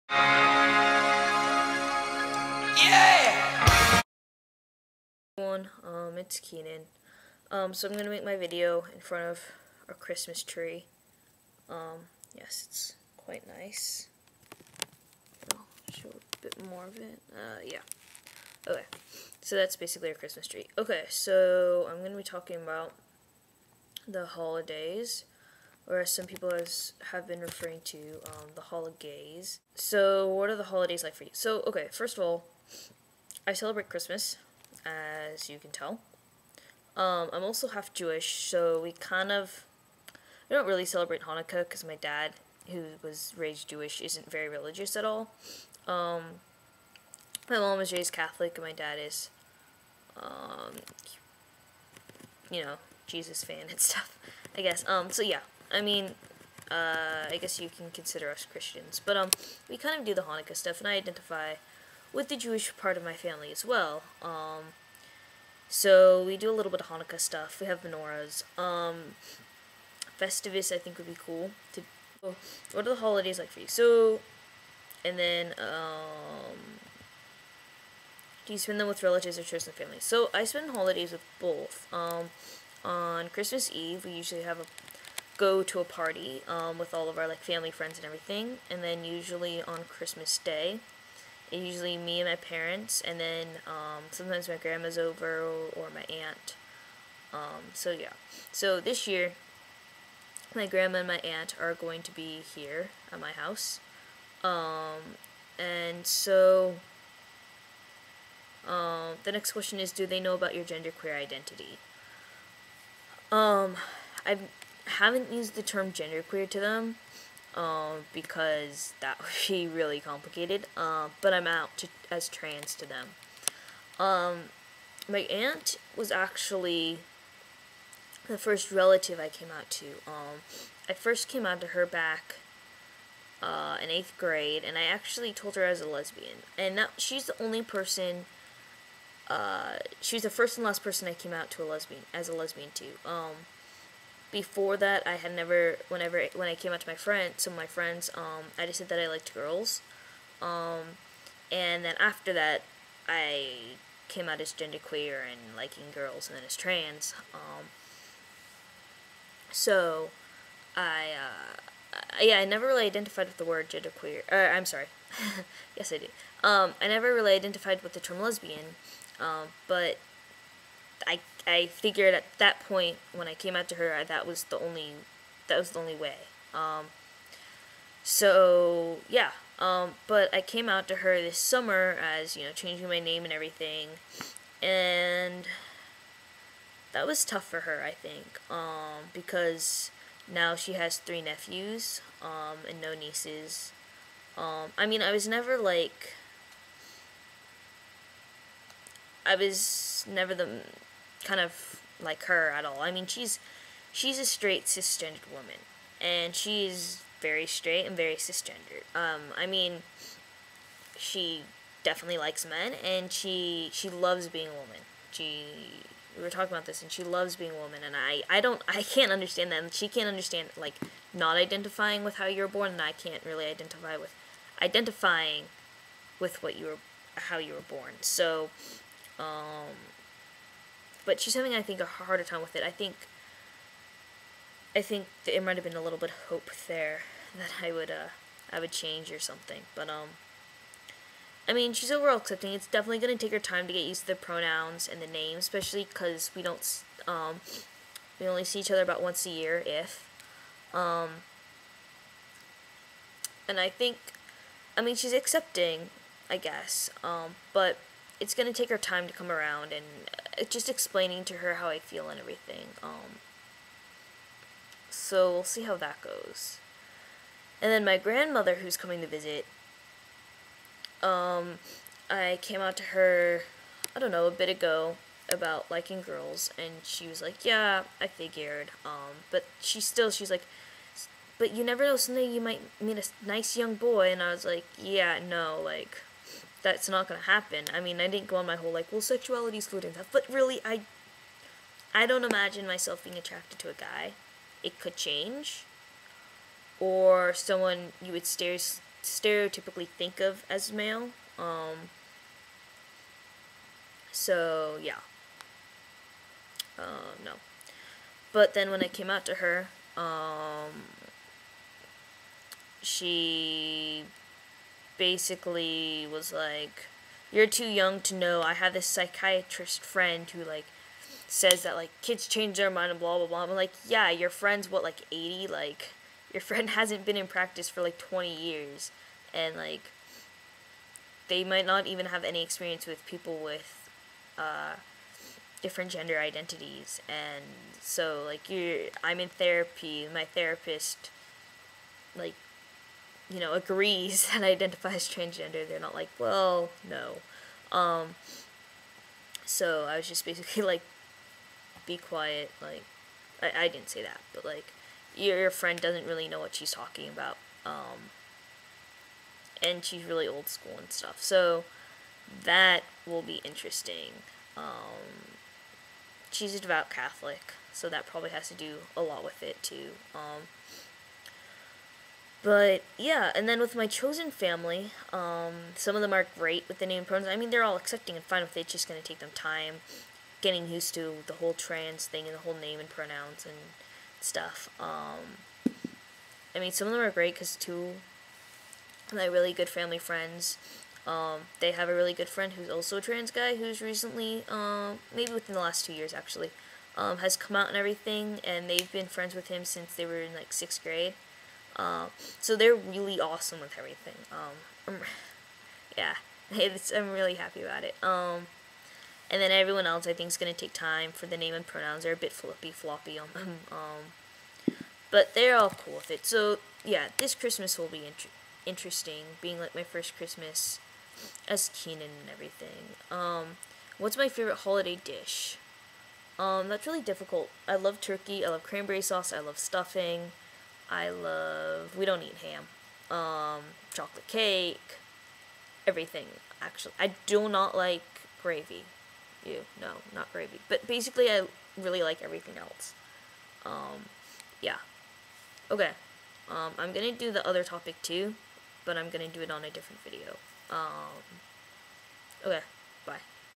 A Genderqueer Holiday
Just talking about being genderqueer and the holidays.